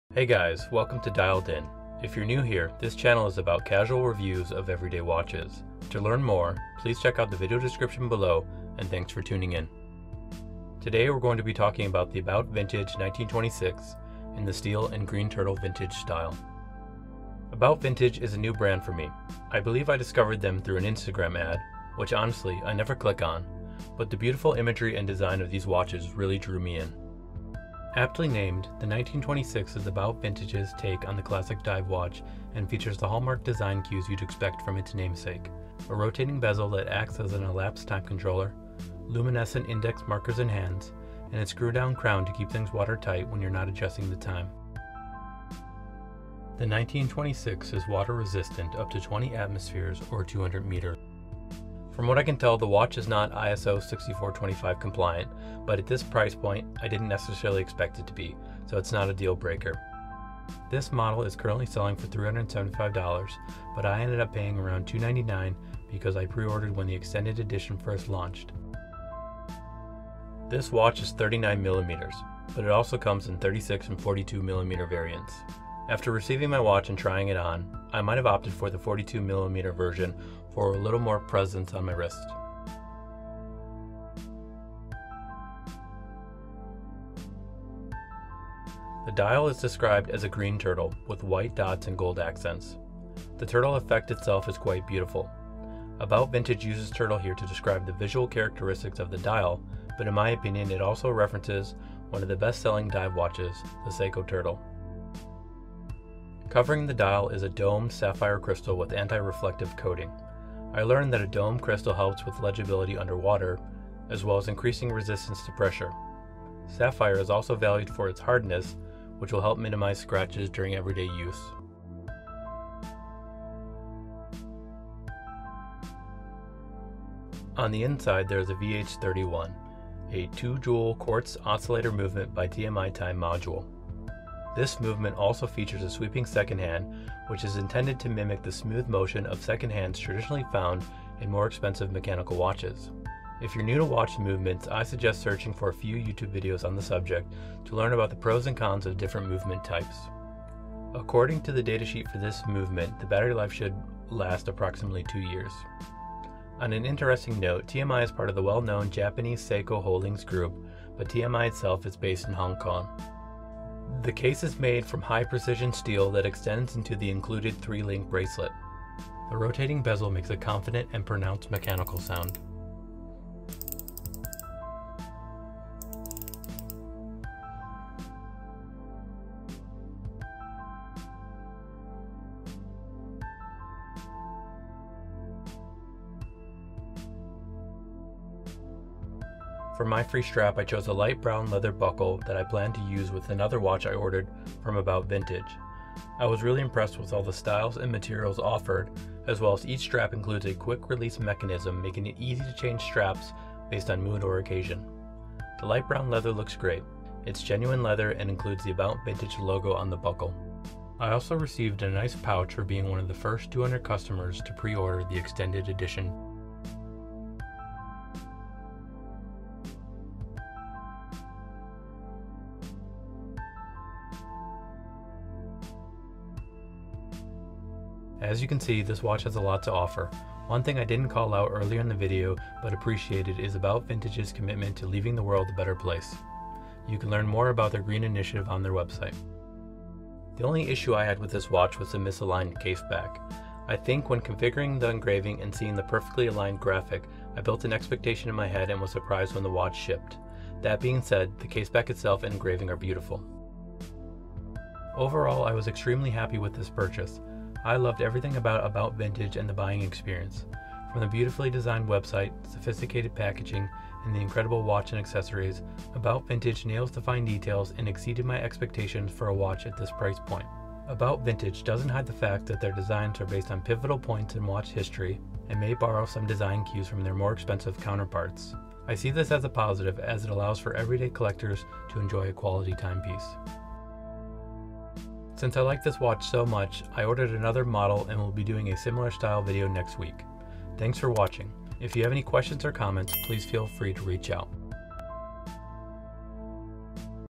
About Vintage 1926 • At' Sea • Steel / Green Turtle Vintage 5 Minute Review
I recently purchased this watch from an Instagram ad and was so impressed with it I decided to make a video review. I see so many watch channels featuring expensive watches and thought it would be cool to do a review of an "everyday" watch in a similar fashion. There are so many people out there who are looking for a nice watch at an affordable price and my hope is this review can help them make a decision.

I hope you guys enjoy this video, please bear with me as this was my first time learning Premiere Pro and I shot all the video on an iPhone and recorded the voice-over in my closet. Thanks all.

Below is the transcript from the video:

Overview:
Today we're going to be talking about About Vintage 1926 in the Steel and Green Turtle Vintage style.

About Vintage is a new brand for me, I believe I discovered them through an Instagram ad, which honestly I never click on, but the beautiful imagery and design of these watches drew me in.

Aptly named, 1926 is About Vintage's take on a classic dive watch and features the hallmark design cues you'd expect from its namesake: a rotating bezel that acts as an elapsed time controller, luminescent index markers and hands, and a screw-down crown to keep things watertight when not adjusting the time.

The '1926' is water-resistant up to 20 atmospheres or 200 meters.

This model is currently selling for $375, I ended up paying around $299.99 because I pre-ordered when the extended edition first launched. In the few months I've been following About Vintage they've always seemed to have some promotion going on.

This watch is 39mm, but it also comes in 36 and 42mm variants. After receiving my watch and trying it on, I might have opted for the 42mm version for a little more presence on my wrist.

The Dial is described as a "green turtle" with white dots and gold accents. The turtle effect is quite beautiful – About Vintage uses turtle here to describe the visual characteristics of the dial, but in my opinion, it also references one of the best-selling dive watches, the Seiko Turtle.

Covering the dial is a domed sapphire crystal with an anti-reflective coating. I learned that a domed crystal helps with legibility underwater, as well as increasing resistance to pressure. Sapphire is also valued for its hardness, which will help minimize scratches during everyday use.

Movement:
On the inside, there is a VH31, a 2 jewel quartz oscillator movement by TMI Time Module. This movement also features a sweeping second hand which is intended to mimic the smooth motion of second-hands traditionally found in more expensive mechanical watches. If you're new to watch movements, I suggest searching for a few YouTube videos on the subject to learn about the pros and cons of different movement types. When browsing the datasheet for this movement I found out that the battery life should be approximately 2 years. On an interesting note, TMI is part of the well-known Japanese Seiko Holdings Group, but TMI itself is based in Hong Kong.

Case:
The case is high precision steel that extends into the included 3 link bracelet. For my free strap, I chose a light brown leather buckle that I plan to use with another watch I ordered from About Vintage. I was impressed with all styles and materials offered, as well as each strap includes a quick-release mechanism making it easy to change straps based on mood or occasion. The light brown leather strap looks great, is genuine leather, and includes the About Vintage logo on the buckle.

ISO:
From what I could tell the watch is not ISO 6425, but at this price point, I didn't necessarily expect it to be, so not a deal-breaker.
What I love about this watch is the subtle details, the gold accents.

Pros:
As you can see this watch has a lot to offer. One thing I didn't call out earlier in the video but appreciated is About Vintages' commitment to leaving the world a better place.

Issue:
The only issue I had with this watch was the misaligned case back – I think when configuring the engraving and seeing the perfectly aligned graphic I built an expectation in my head and was surprised when the watch shipped. That being said, the case back itself and engraving are beautiful.

Closing Thought:
Overall I was extremely happy with this purchase - I loved everything about About Vintage and the buying experience. From the beautifully designed website, sophisticated packaging, and the incredible watch and accessories, About Vintages nails the fine details and exceeded my expectations for a watch at this price point. About Vintage doesn't hide the fact that their designs are based on pivotal points in watch history and may borrow some design cues from their more expensive counterparts – I see this as a positive as it allows for everyday collectors to enjoy a quality timepiece.